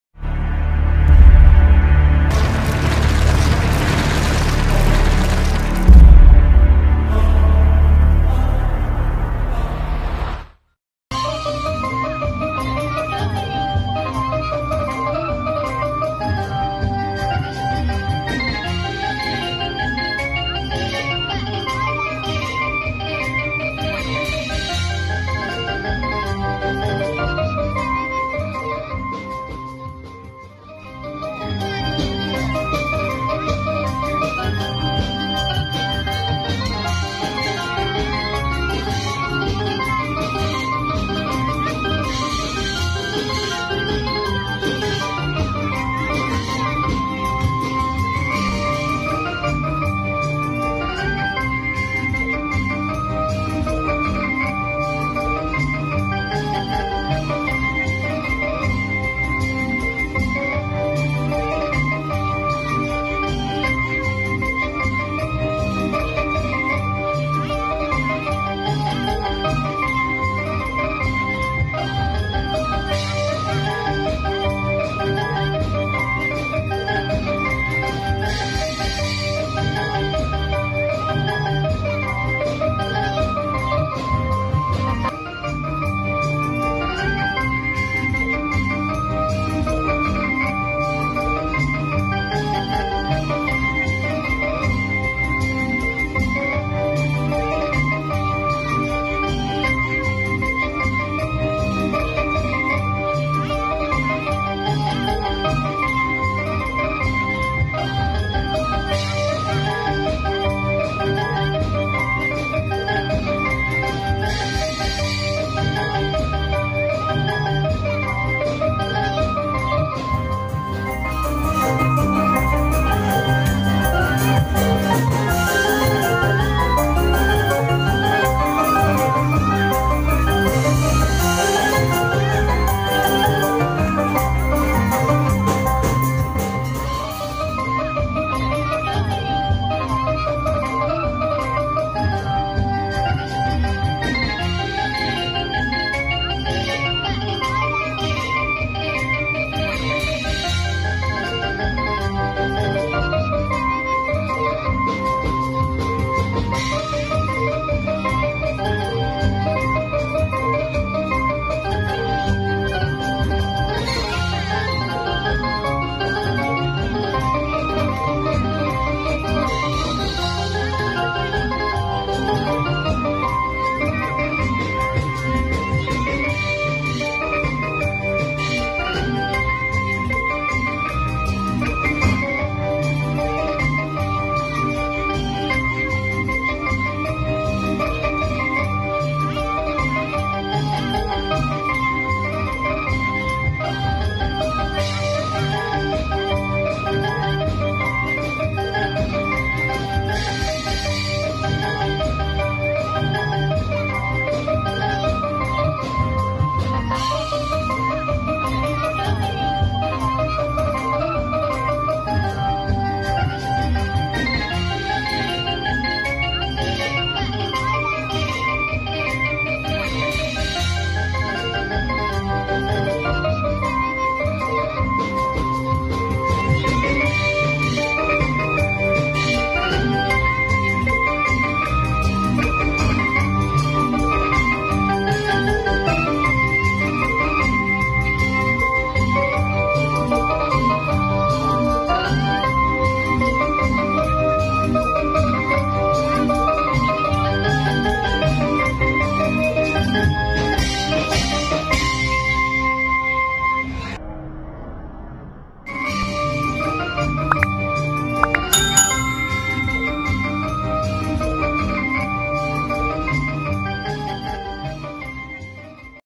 Heboh ‼️Barongan Sambeng Hanoman & Celeng Bikin Geger Jagat Raya
Barongan Sambeng Hanoman keluar dari sarangnya dan menggemparkan warga sekitar, anak anak dewasa orang tua berbondong bondong menyaksikan atraksinya, barongan Sambeng menghebohkan warga sekitar, karya seni dan kreasi anak bangsa Cirebon Jawa barat desa Sambeng kecamatan gunung jati, seni budaya Cirebon desa Sambeng kecamatan gunung jati, hiburan anak anak, terimakasih semoga Vidio ini menghibur semuanya..